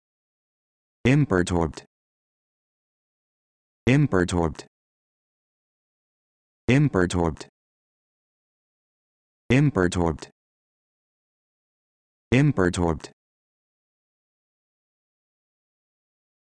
How to Pronounce Imperturbed
This video shows you how to pronounce Imperturbed